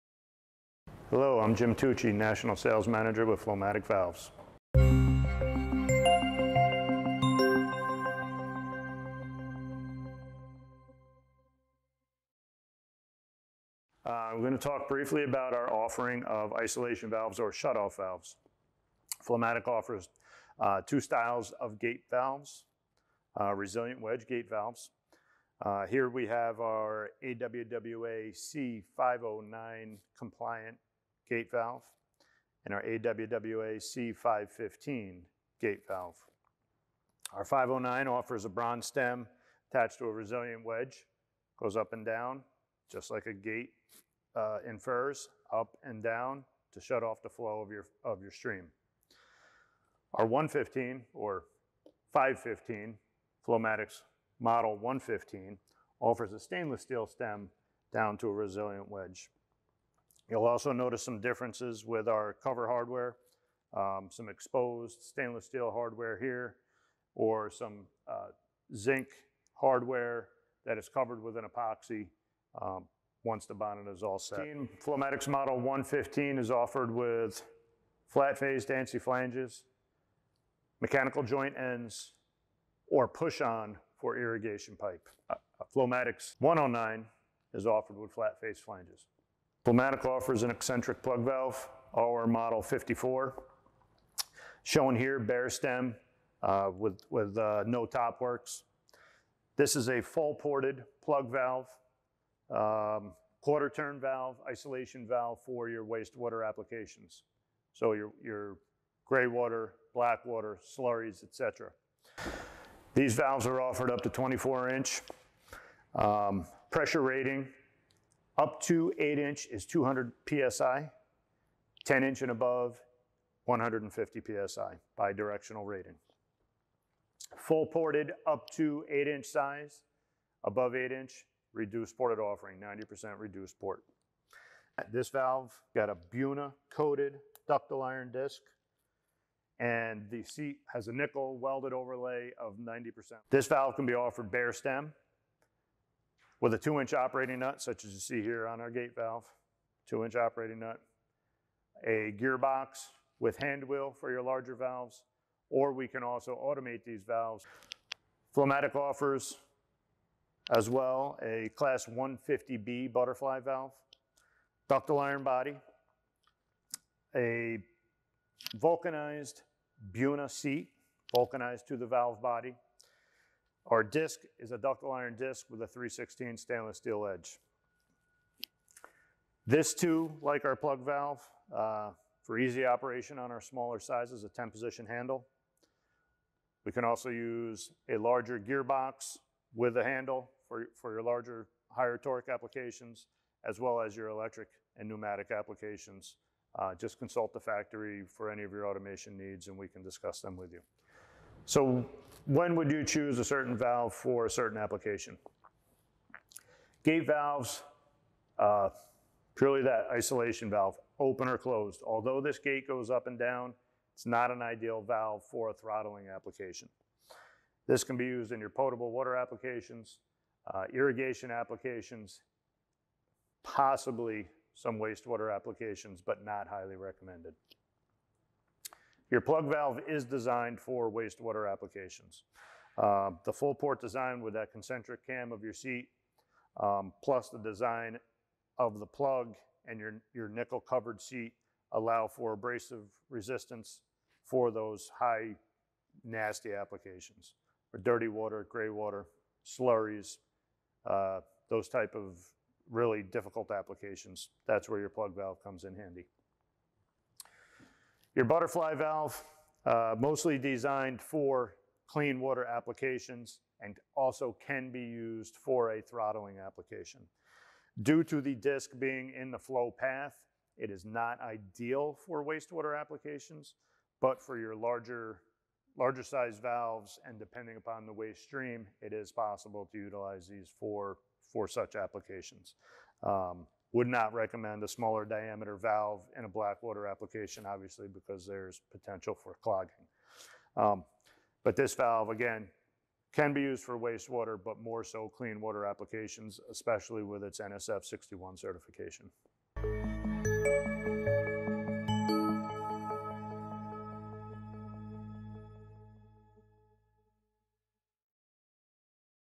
Shutoff Valves by Flomatic: When & Where to Select a Butterfly Valve, a Gate Valve, or a Plug Valve
As their name suggests, shut-off valves are designed to ultimately stop fluid flow or throttle it back to achieve desired flow parameters. These mechanisms serve a vital role in system function and are all too often overlooked when it comes to essential components.

All shut-off valves are built to stop or slow water at a specific point in a pipeline. However, variations in flow rates, pressure, pipe diameters and fluid properties all play a role in determining the design of valve needed. With various styles of shut-off valves available, becoming knowledgeable in each one and their intended uses will help you decide which valve is correct for your application.